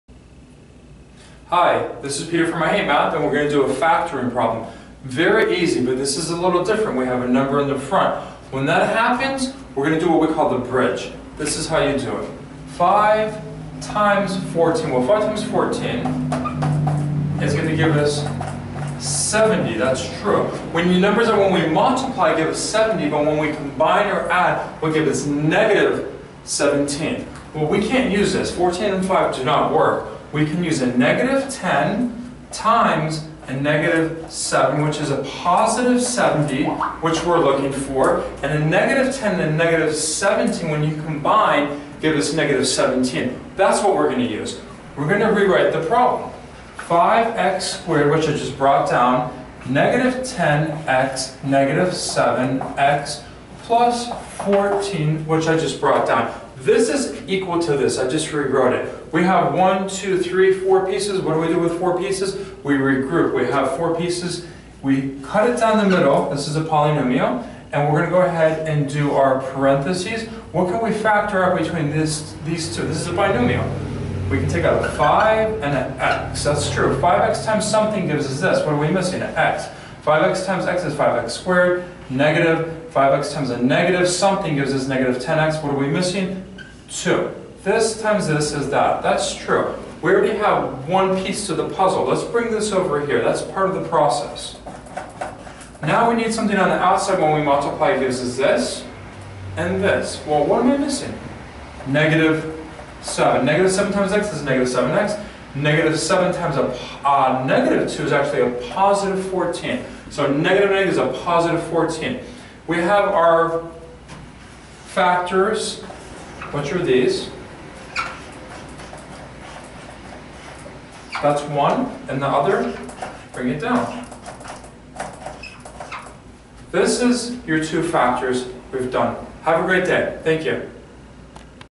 Pert Practice factoring
Hi Guys, this video will review factoring for the Pert test !